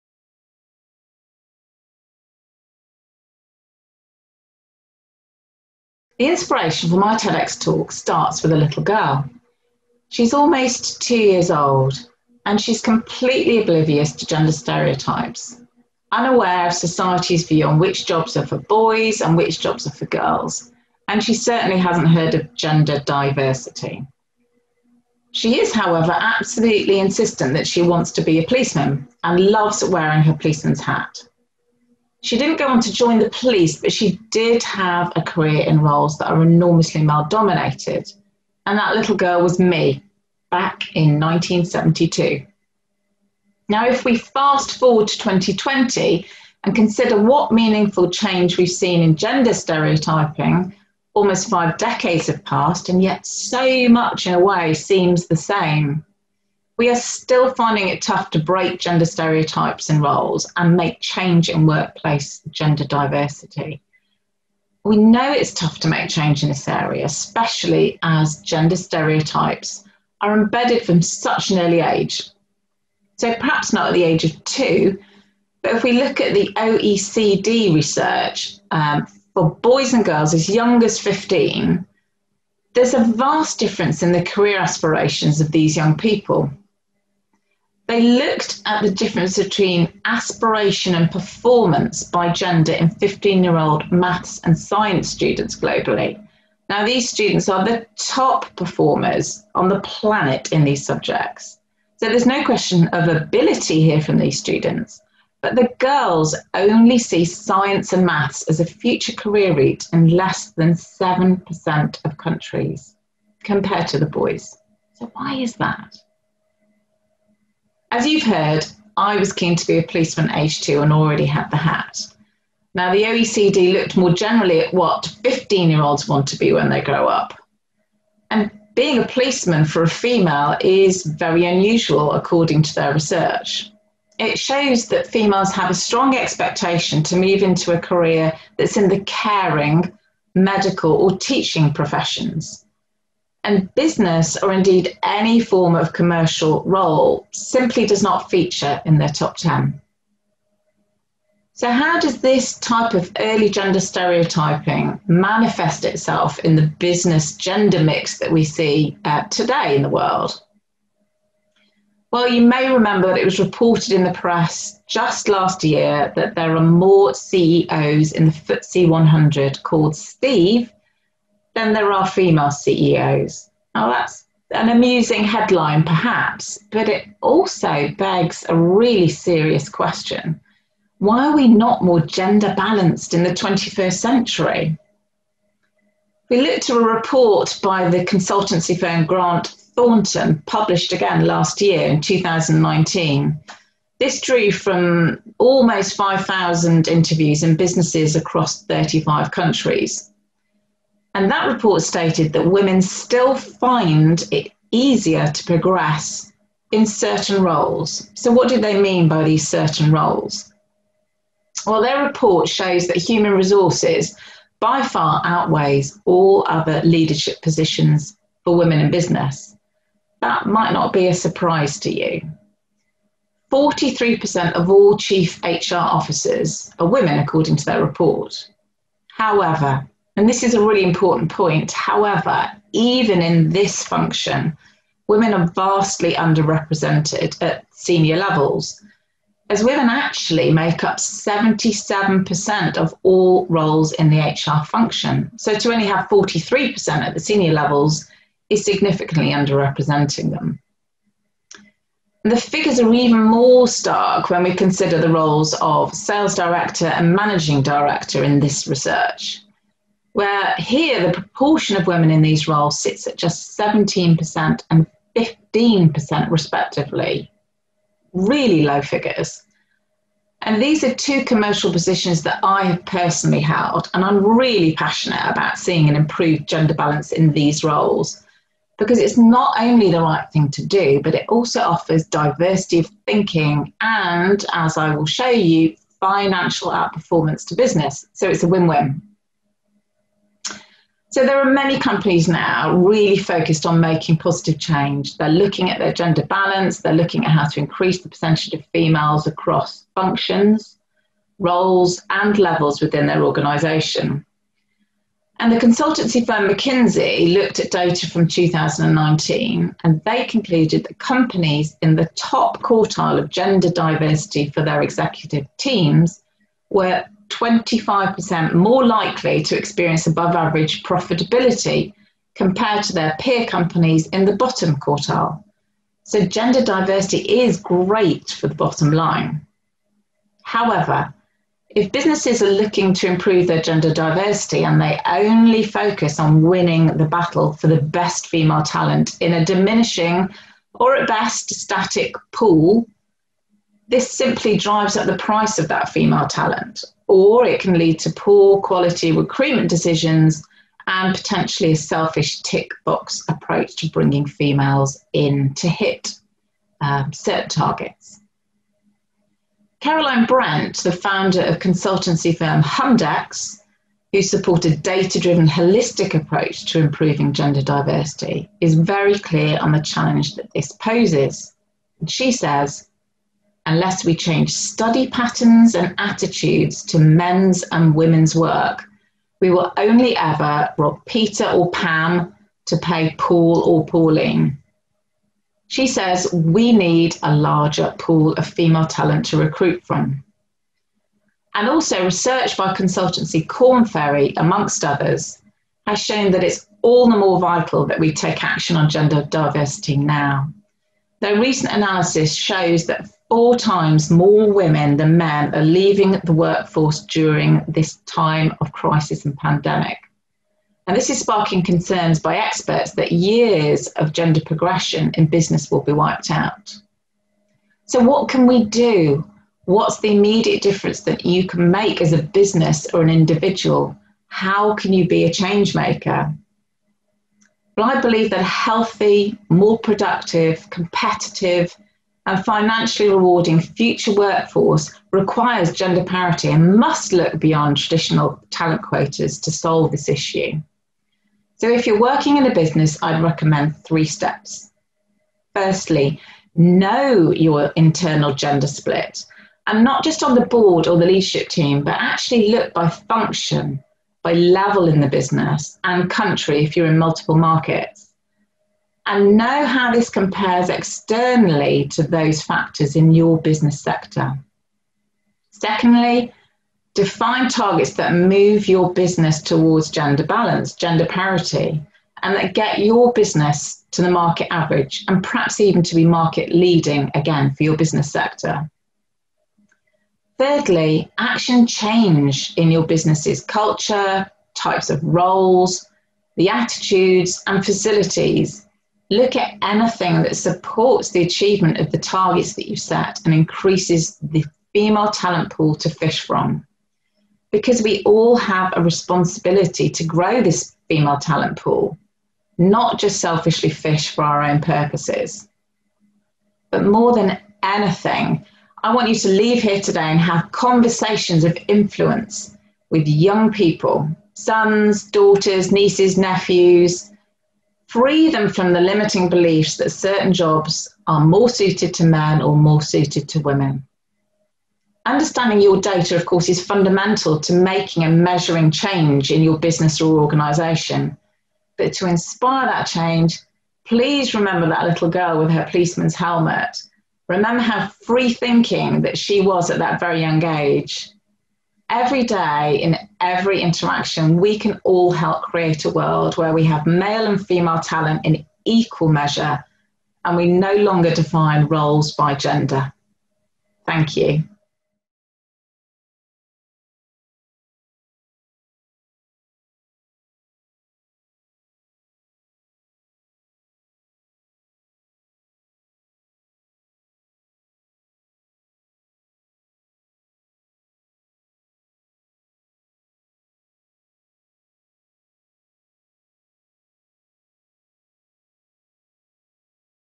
Breaking The Selfish Cycle On Gender Diversity | Thea Roberts | TEDxWolverhampton
In this inspirational talk Thea explores gender stereotypes and the importance of diversity in business. She examines the need for greater gender balance and appeals to businesses to grow their female talent pool – not just selfishly fish for their own purposes. Thea’s experience spans nearly 30 years in commercial leadership roles up to MD level, with responsibility for turnover in excess of £450M and teams over 100.

She has led and developed large teams for retail and
consumer packaged goods corporations, including Mars, Marks & Spencer, Waterstone’s and Essity.

Fascinated and motivated by facilitating positive change, and unlocking potential in individuals and teams, she started her own business as an Executive Coach, Mentor and Speaker in 2018. This talk was given at a TEDx event using the TED conference format but independently organized by a local community.